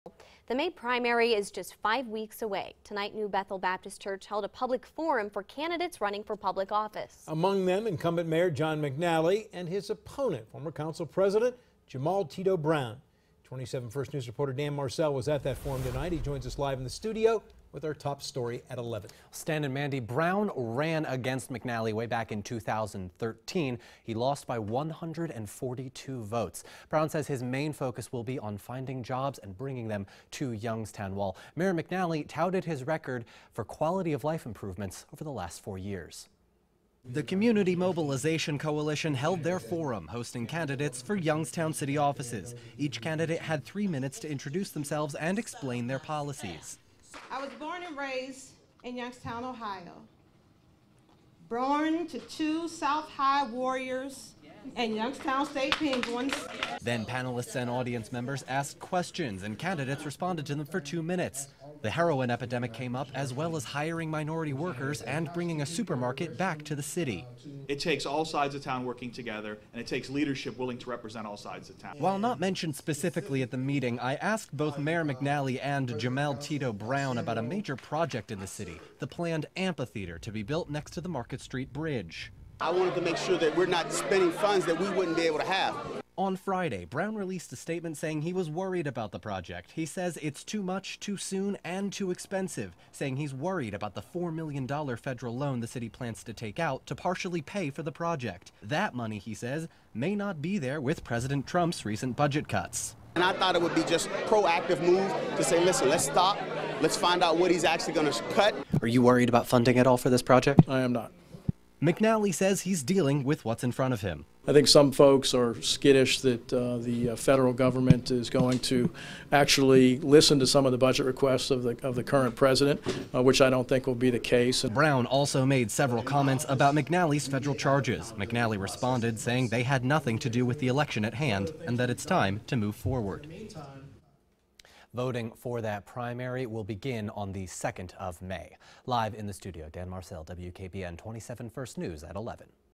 Youngstown mayor, opponent disagree on downtown amphitheater plans
Jamael Tito Brown is concerned about federal money to pay for the project being cut, but Mayor John McNally isn’t worried.